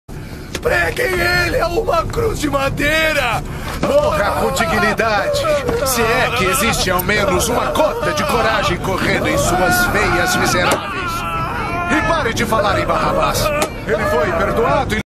Rebel nailed to the cross
Crucifixion, an important method of capital punishment particularly among the Persians, Seleucids, Carthaginians, and Romans from about the 6th century BCE to the 4th century CE. Constantine the Great, the first Christian emperor, abolished it in the Roman Empire in the early 4th century CE